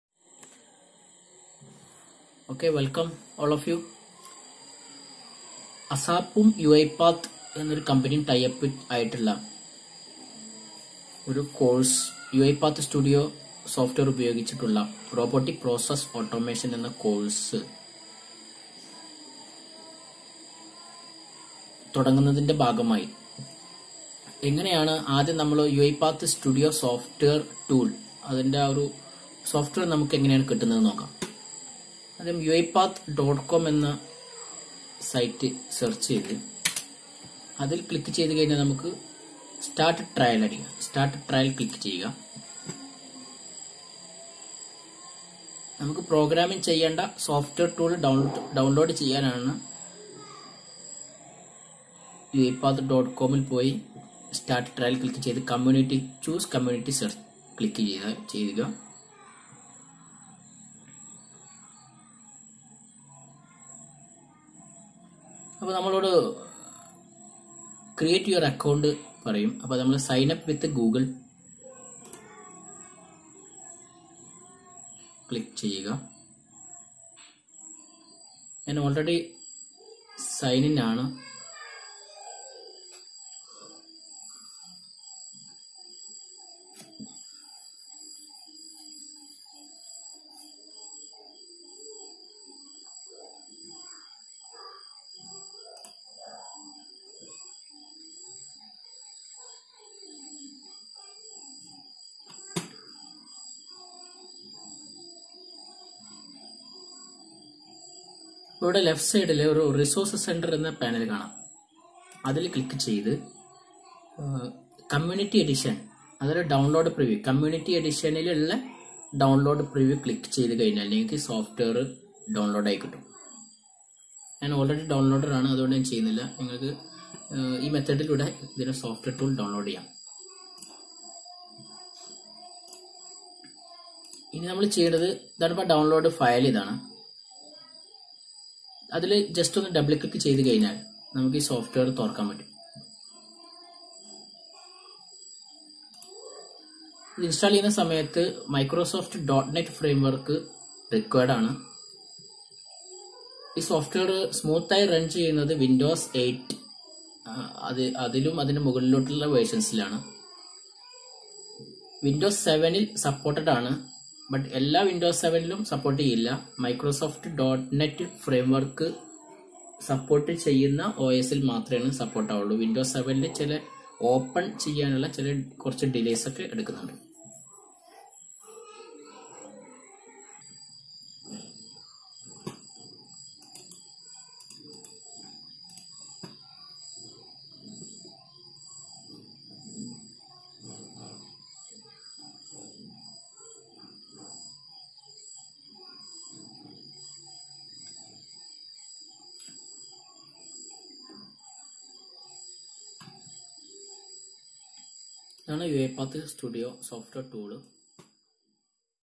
UiPath tutorial 1 (malayalam)
how to get Uipath studio software